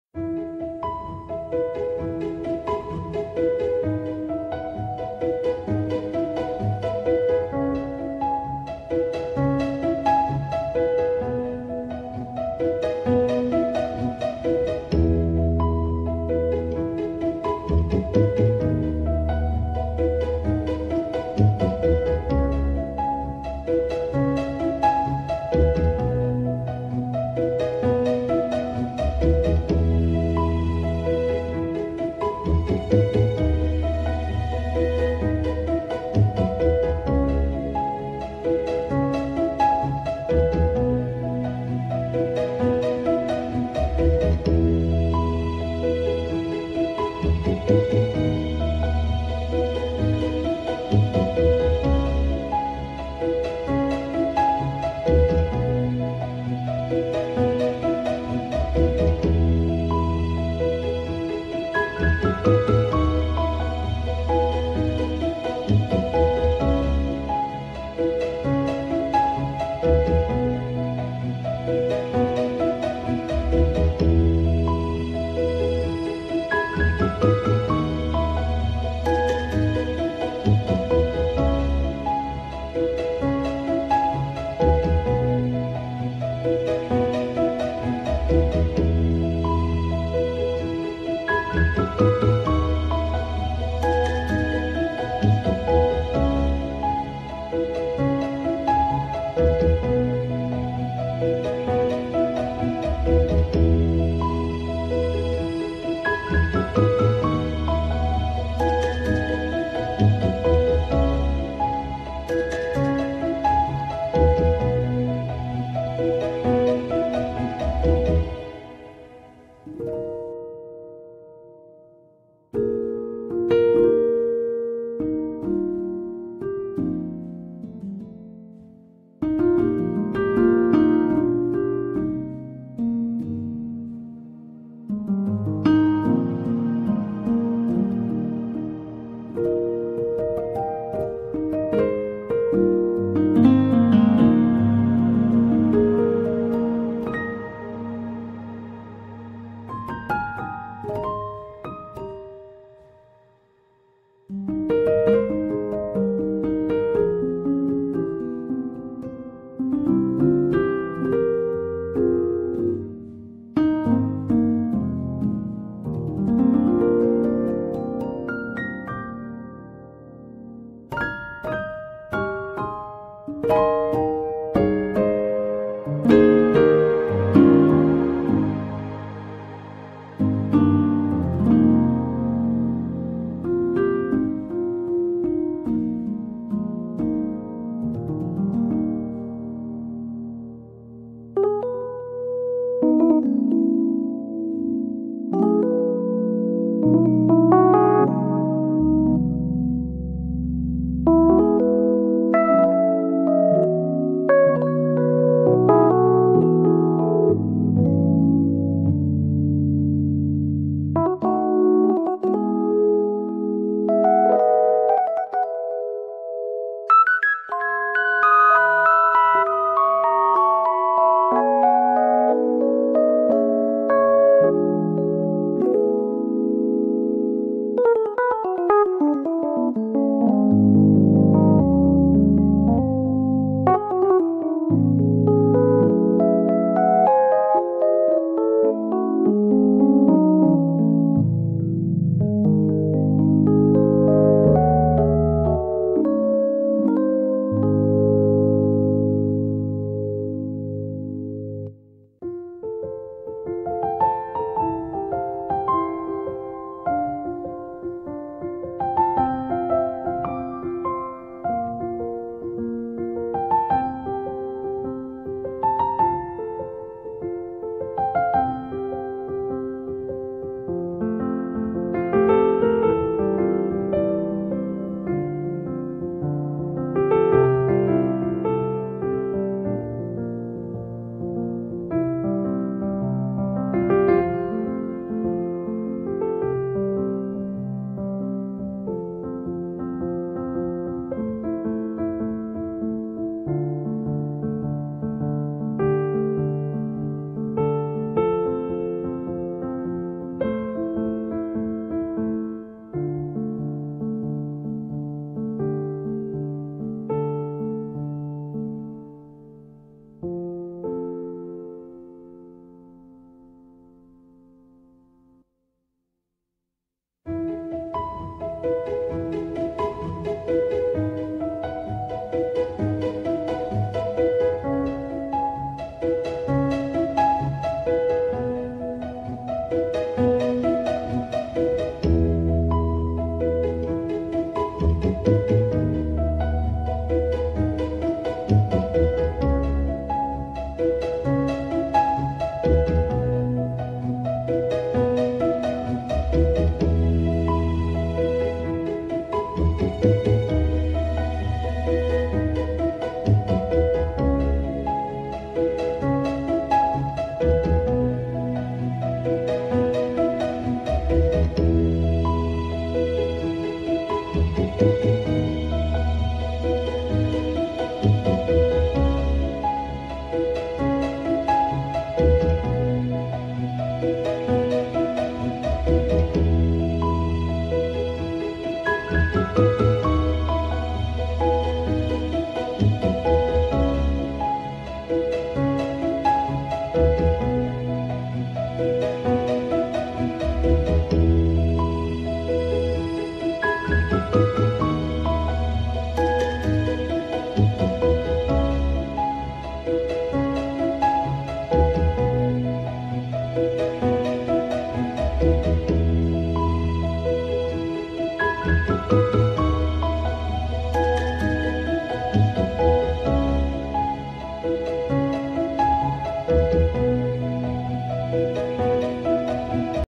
SD Santo Yoseph 1 Medan (Doa Pagi Pukul 06.00 Wib di Kapel)
Retret Gelombang Pertama Kelas 6 A & 6B SD Santo Yoseph 1 Medan